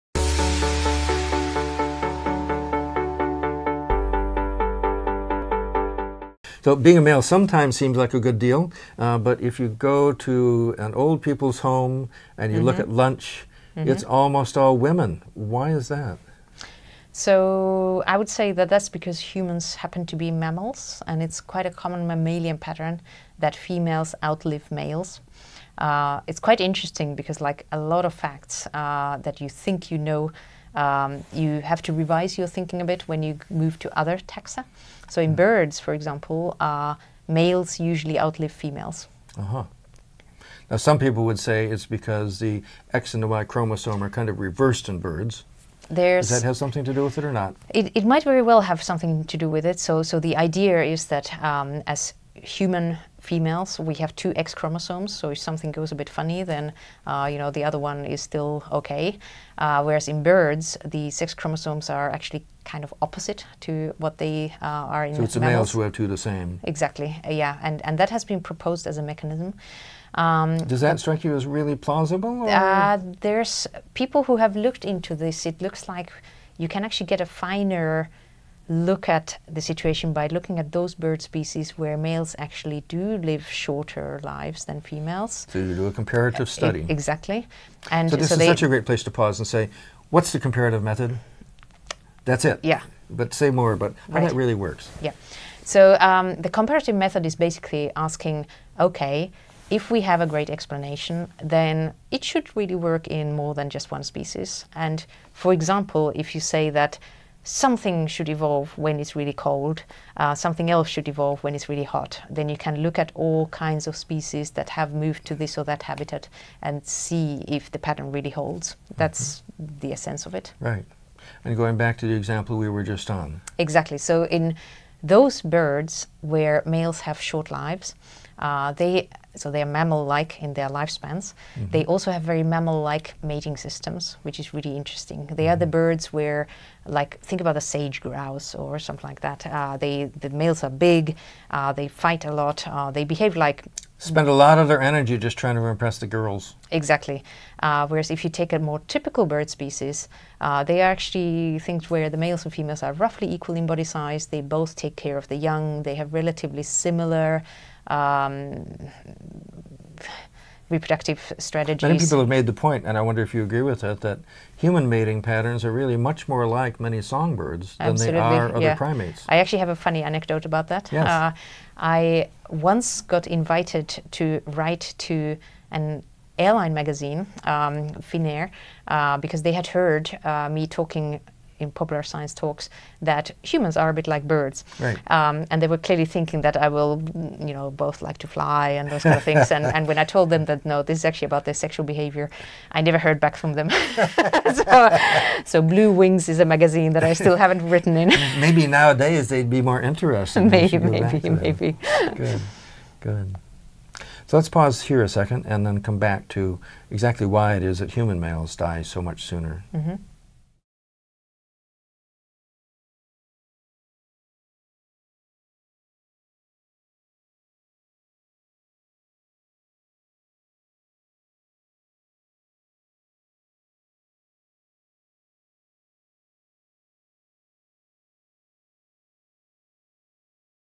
Male and Female Lifespan | Hanna Kokko
Hanna Kokko has a longstanding interest in the mathematical logic that underpins biology, with a particular curiosity towards the evolution of reproductive strategies. She finds everything captivating as soon as it involves conflict and possible cooperation between entities (individuals, genes, parts of society...) and this then leads to demographic consequences within or across species.